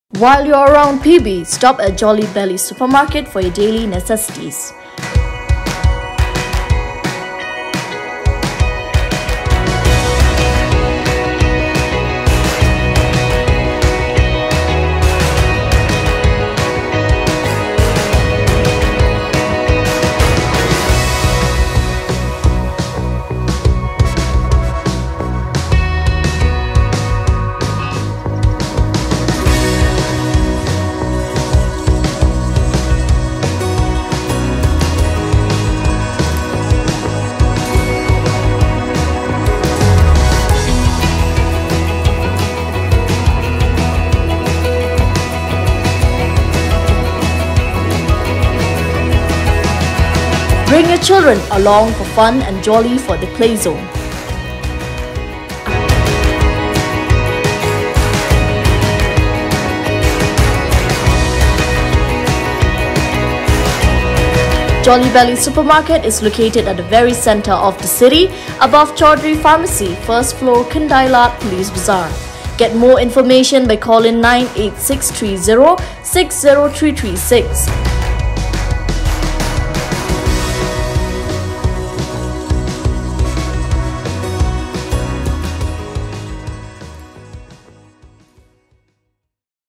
Visit Jolly Belly Supermarket for your daily requirements
Visits Jolly Belly Supermarket for your daily requirements, bring your kids along for fun and jolly in at the Play Zone within the supermarket itself. located at the heart of the city, above Choudhury Pharmacy, first floor, khyndailad, police bazar.